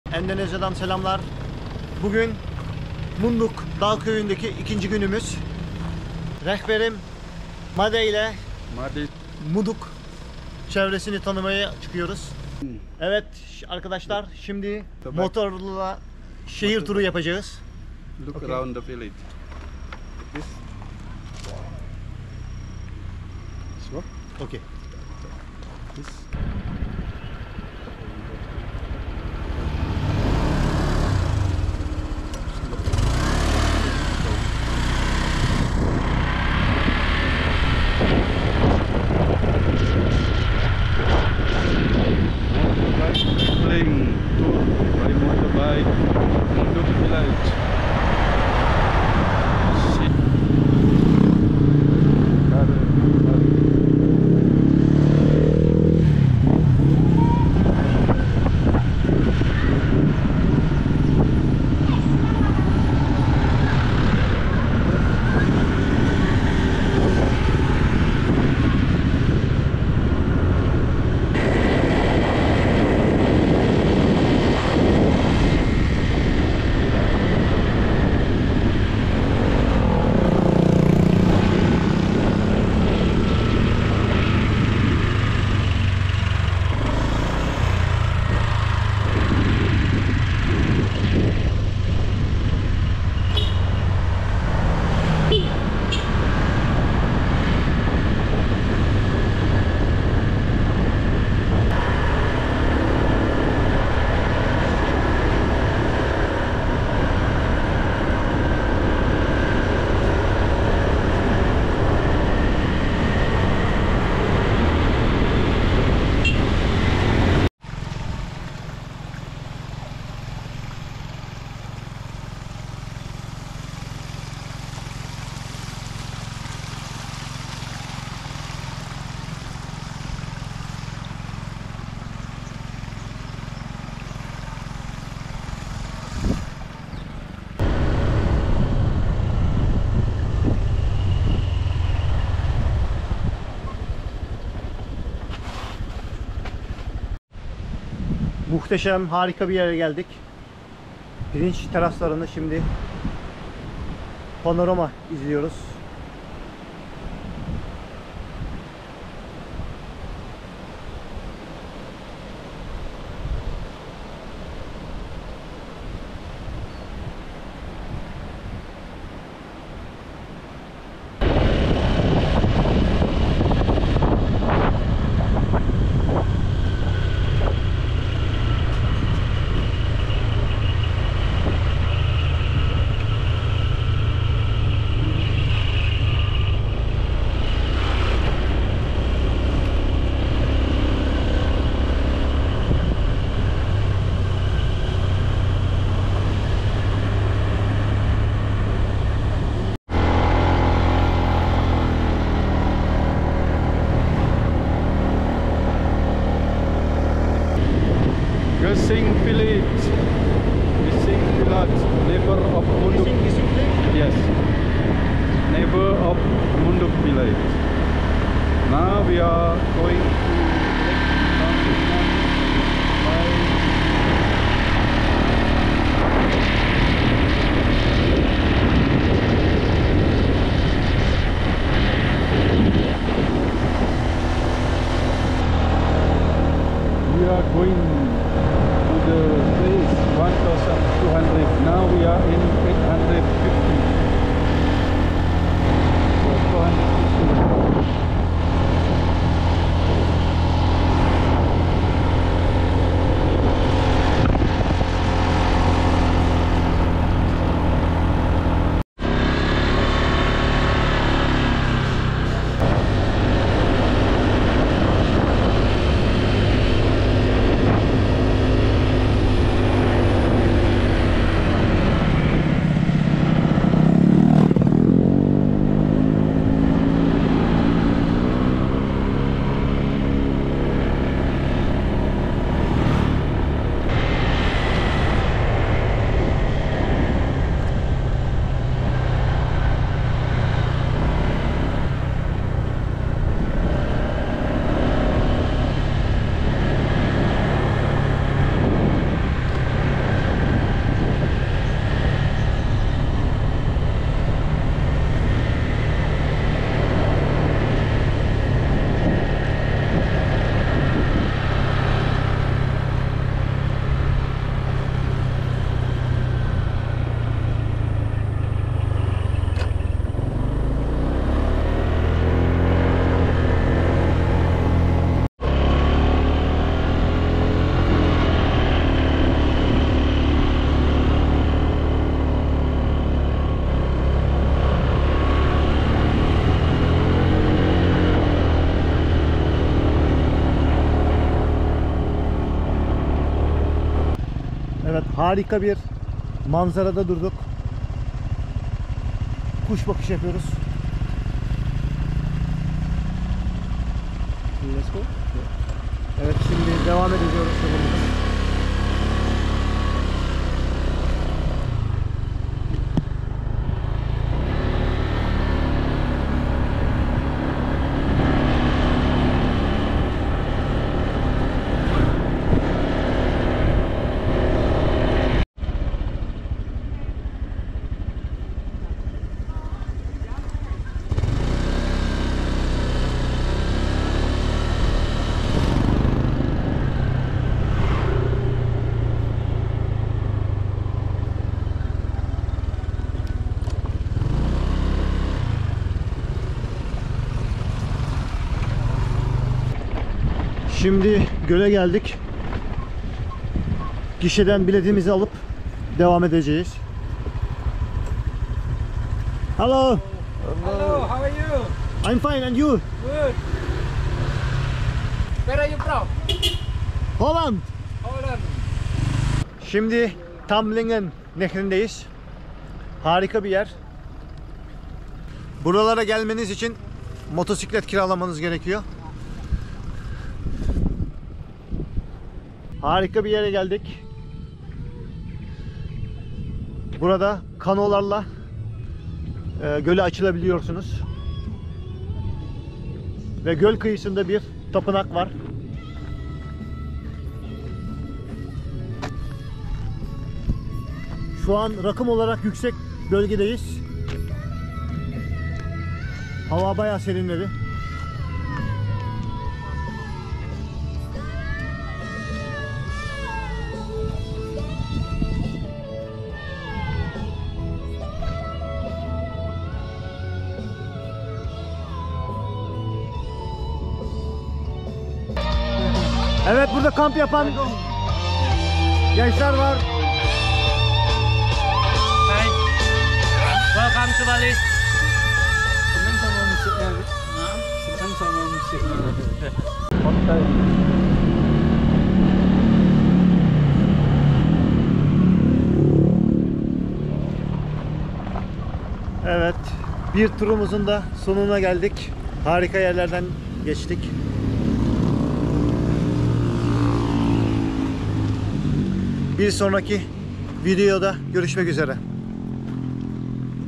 Bali Adası Munduk dağ köyü çevresini motorla gezdim
Bali Adası’nda kiraladığım motor ile Munduk dağ köyü çevresini gezdik. Made Sawika beni motoruyla Munduk Köyü çevresindeki turistler tarafından pek bilinmeyen yerlere götürdü. Yol boyunca kayıttaydım. Yolumuzun üstünde sık sık mola verdik. Yol boyu tablo gibi manzarası olan pirinç teraslarından geçtik. Bölgenin rakımı yüksek yerinde yer alan bir doğa harikası olan Tamblingan Gölü’nün muhteşem tabiatı ise kesinlikle görülmeye değerdi.